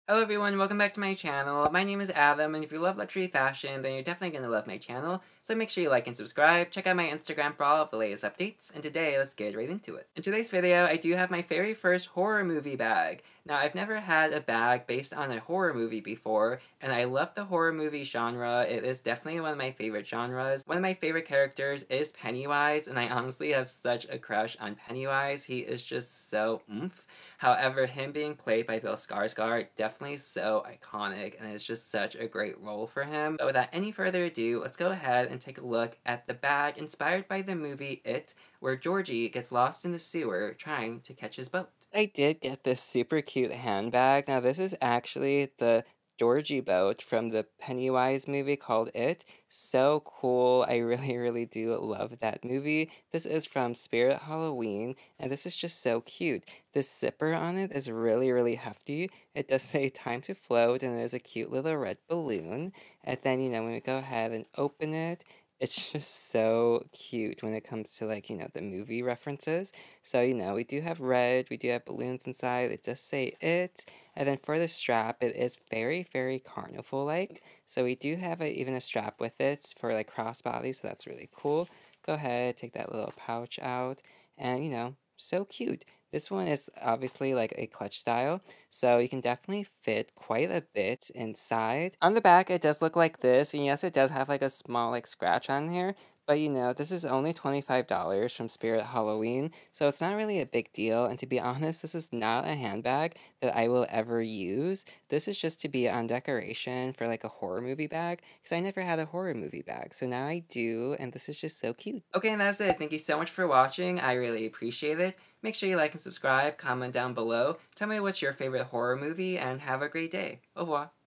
HORROR MOVIE HANDBAG? Novelty Handbag Unboxing!
Hi, I'm Adam!

Today let’s check out a bag based on the Horror Movie, "It"! Pennywise is one of my favorite characters in the horror movie genre, so when I seen this I had to get it! Georgie's Boat will make a cute decoration piece on my Luxury Handbag Shelf.